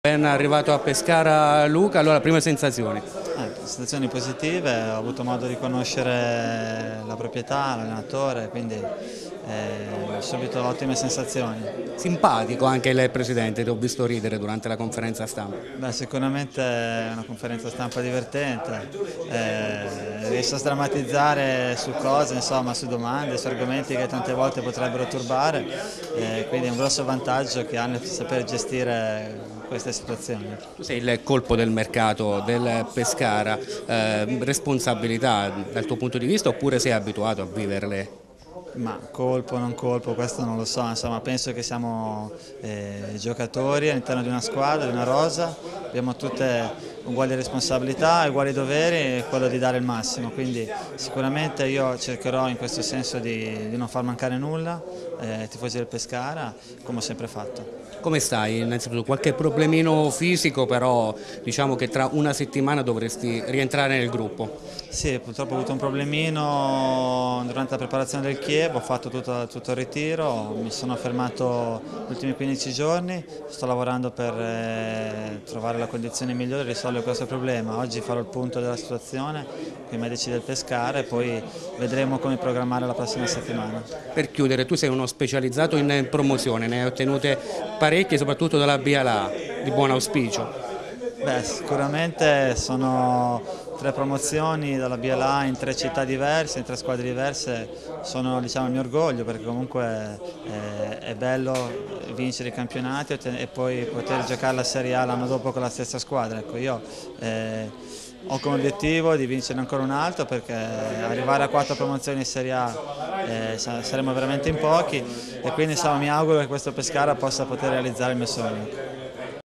Luca Ariatti, specialista in promozioni
Microfono a Luca Ariatti, il "colpo" del mercato biancazzurro. 32 anni, arriva dal Chievo ed è un autentico specialista in promozioni